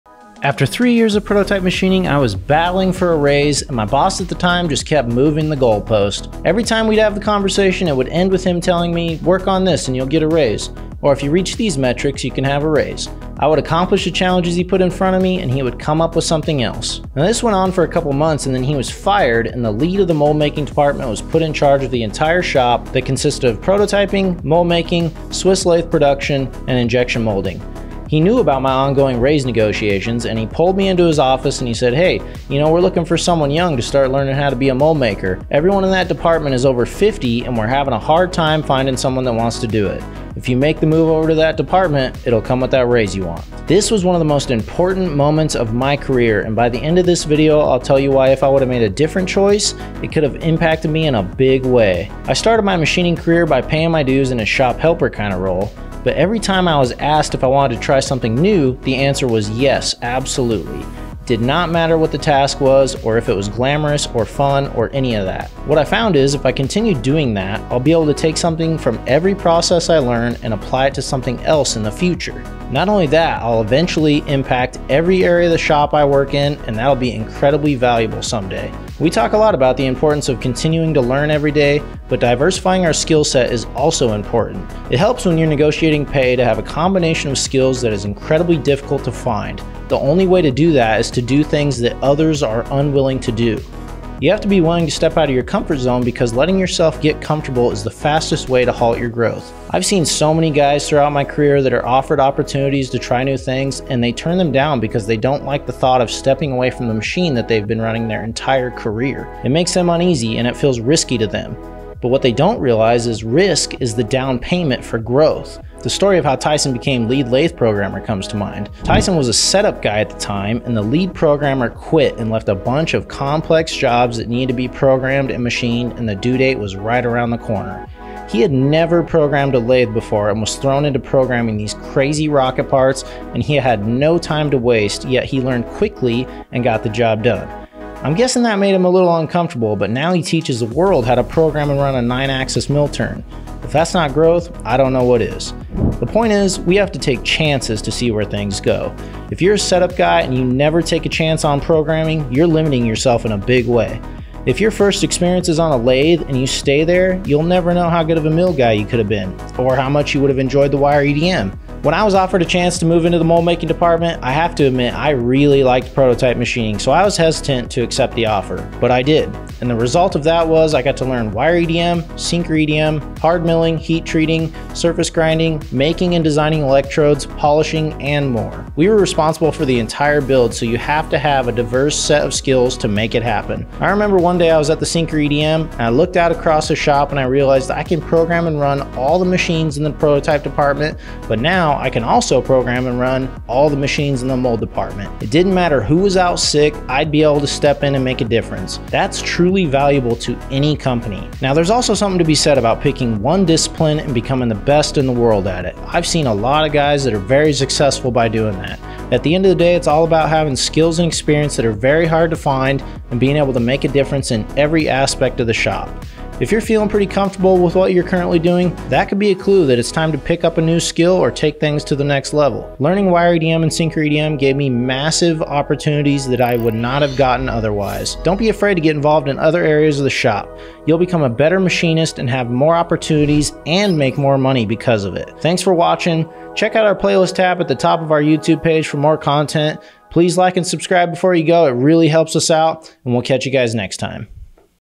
Most People Have No Clue or Simply Don’t Understand How Important This Is
Trevor gives insight to what it took for him to achieve a higher status in his shop, and become an all around asset to the team.